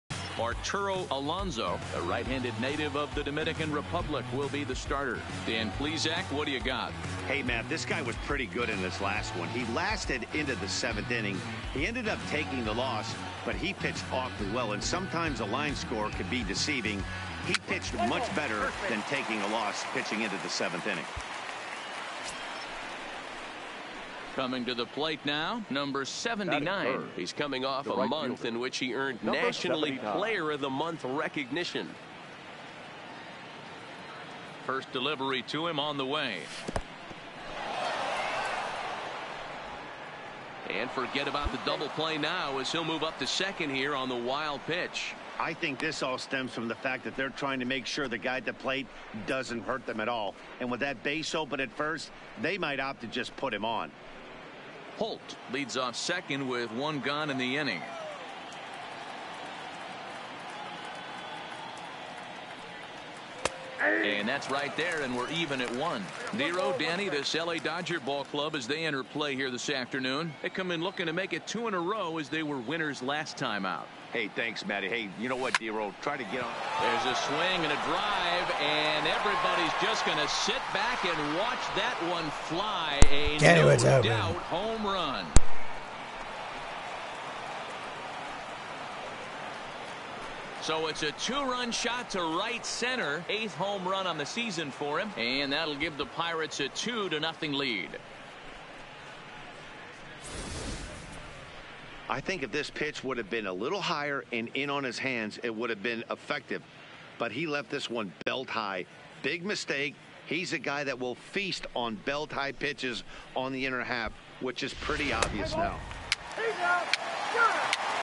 E:691 19-5-1 - Super Joe Loves the LA Sun! Home Run Blast! (439 feet)(S 8)(C 268) v. Dodgers
E:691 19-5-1 - Pirates (15-11) @ Dodgers (19-10) - SuperJoe - Super Joe loves the Los Angeles Sun! Home Run Blast! (439 feet)(S 8)(C 268). Joe's 2-4, 3B (3)(C 67), HR (8)(C 268), 2 RBI (25) led the Pirates to the 4-0 win as he raised his average to .410. Ian Kennedy was strong as he picked up his 4th win of the season (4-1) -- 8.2 /5/0/0/2/10.